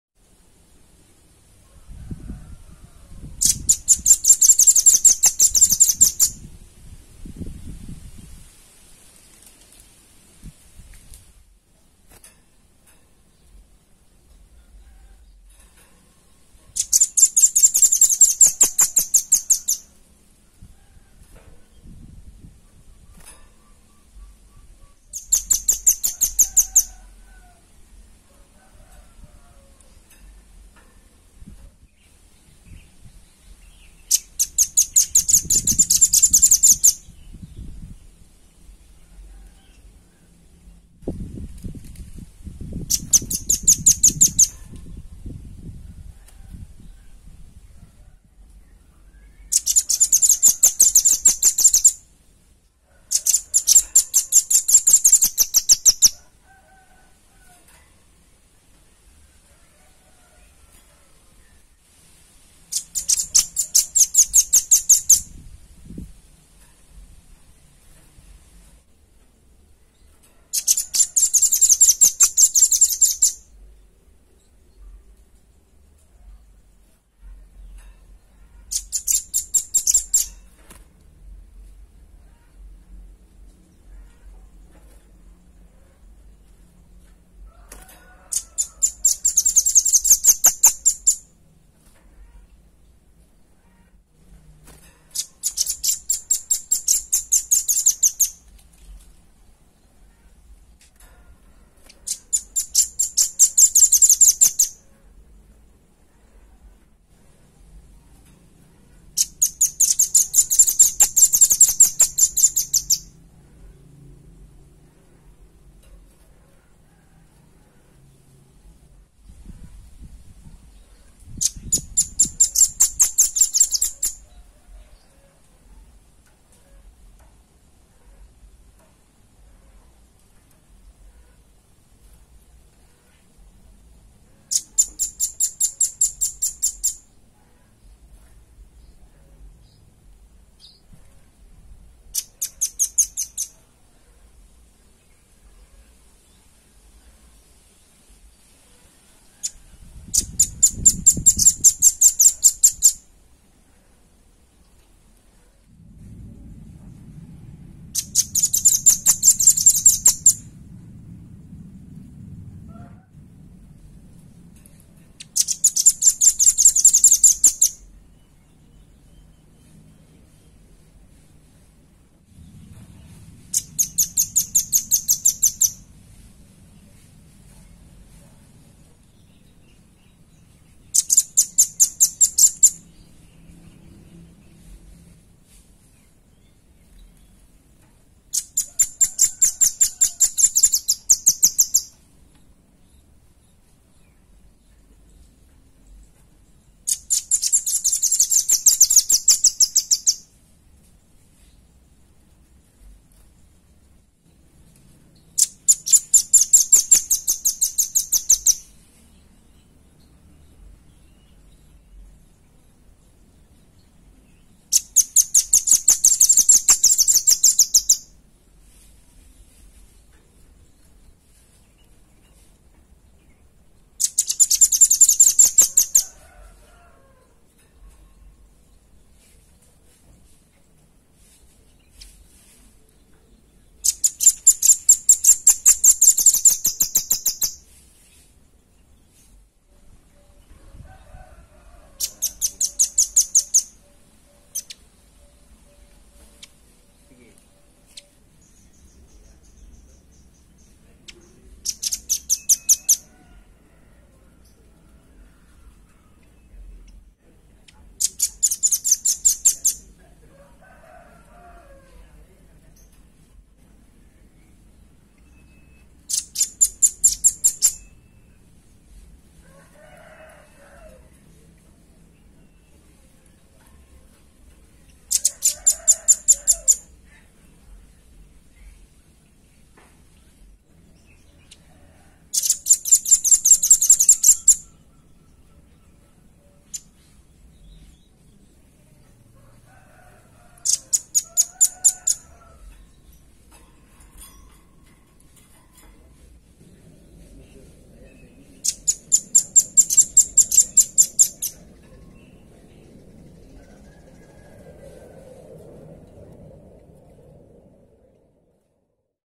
SUARA PANCINGAN & MASTERAN RAMBATAN PARUH MERAH JERNIH !!!
suara rambatan paruh merah jernih khusus untuk pancingan & masteran.